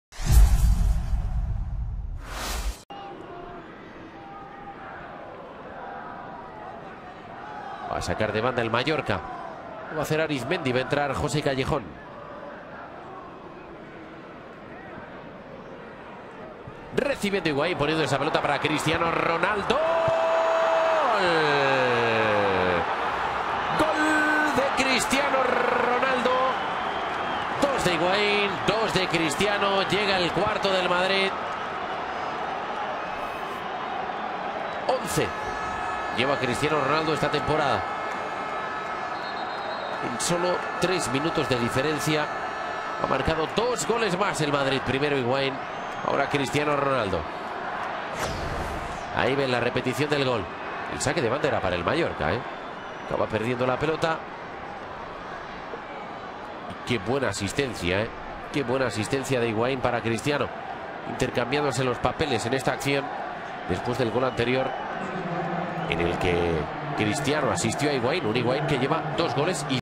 La Liga | Gol de Cristiano Ronaldo (0-4) en el Mallorca - Real Madrid | 28-10-2012 | J9
SUSCRIBITE AL MEJOR CANAL HD La Liga | Gol de Cristiano Ronaldo (0-4) en el Mallorca - Real Madrid | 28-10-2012 | J9 Gol de Cristiano Ronaldo en el RCD Mallorca - Real Madrid (0-5) J9 Liga BBVA 2012/2013